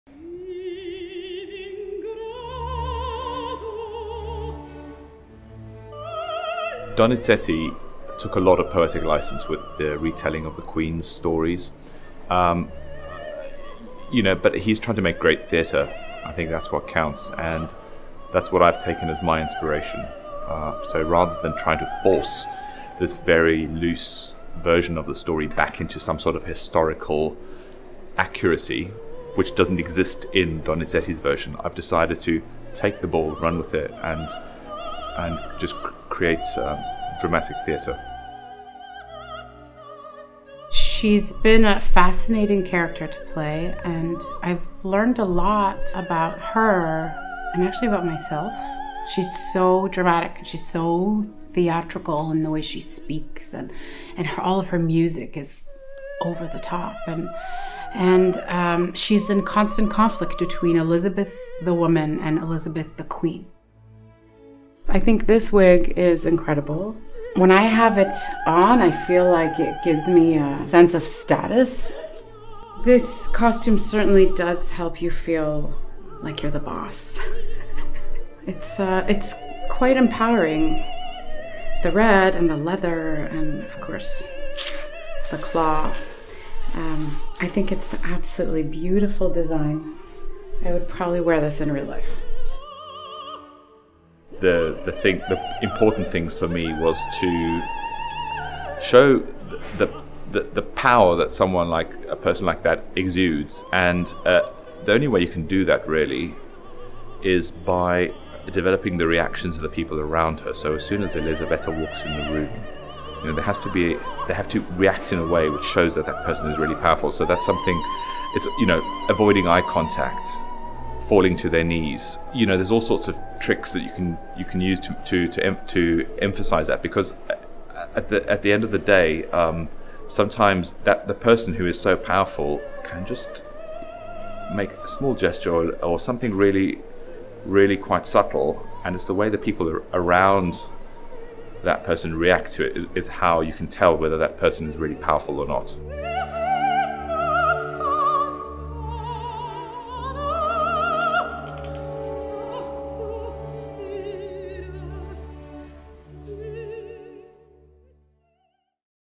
Dod yn Frenhines Elisabeth I | Becoming Queen Elizabeth I
A story of love, power, vengeance and tragedy with a fearsome Queen at its core.
What does it take to become such a formidable ruler? Welsh National Opera goes behind the scenes with Joyce El-Khoury to see how she prepares for such a challenging role.
Stori am gariad, pŵer, dial a thrychineb gyda Brenhines ddychrynllyd yn ei graidd.
Beth mae'n ei gymryd i fod yn rheolwr aruthrol? Mae Opera Cenedlaethol Cymru yn mynd y tu ôl i'r llenni gyda Joyce El-Khoury i weld sut mae hi'n paratoi ar gyfer rôl mor heriol.